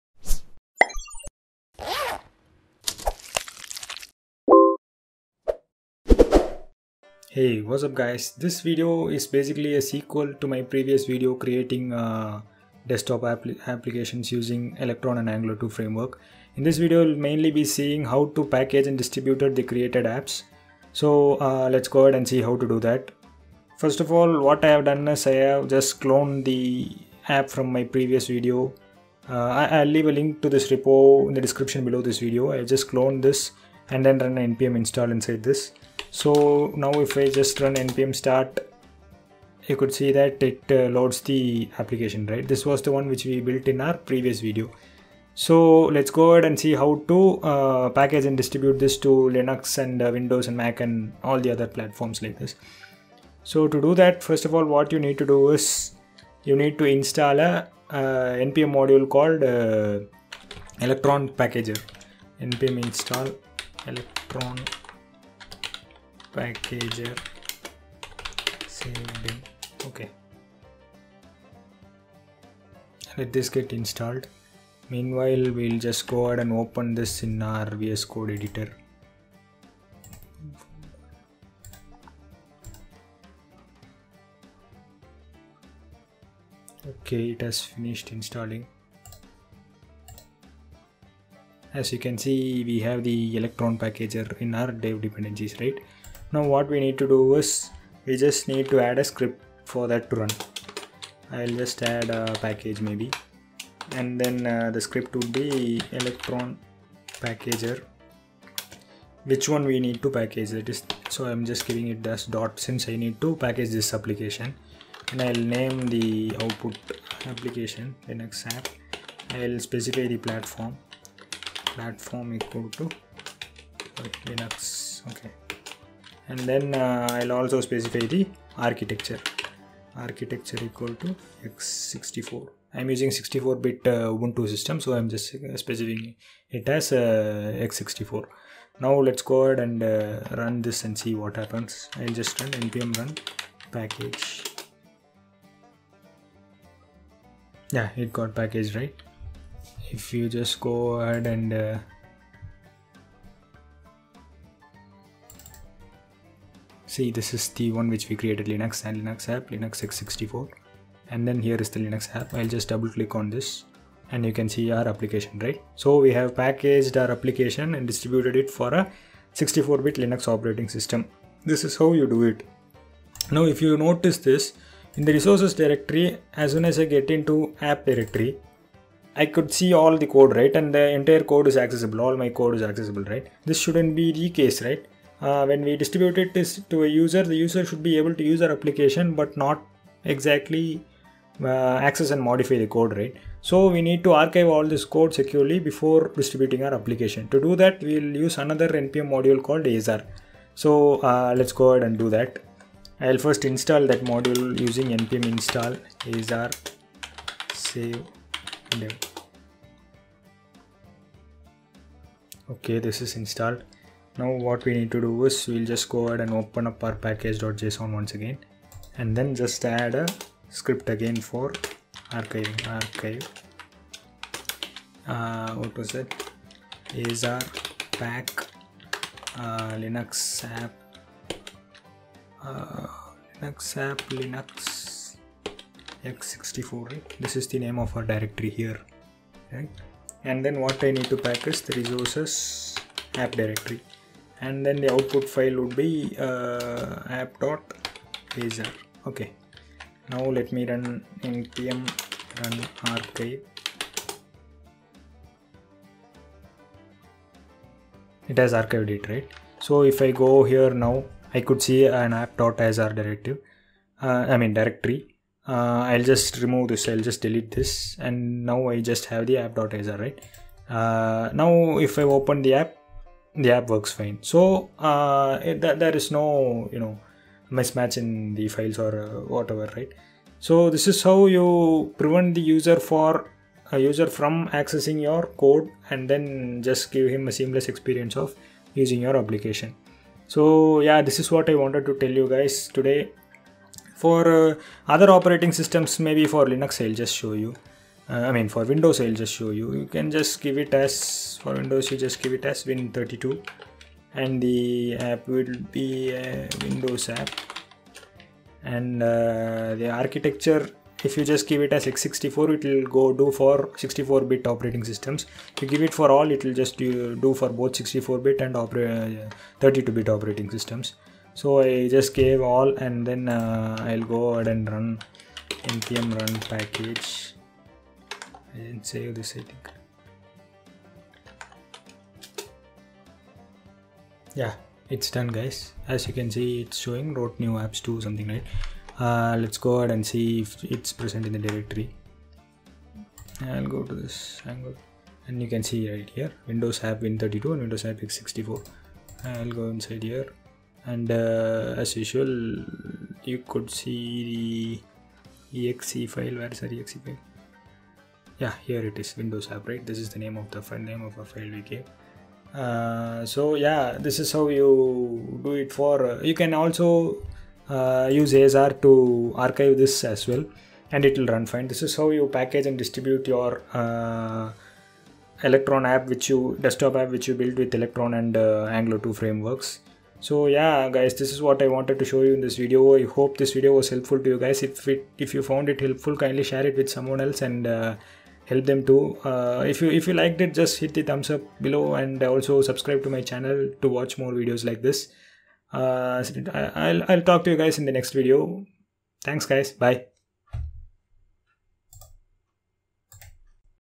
Packaging and distributing your electron/angular 2 desktop apps.
Packaging and distributing our desktop apps created with electron & angular 2. For further details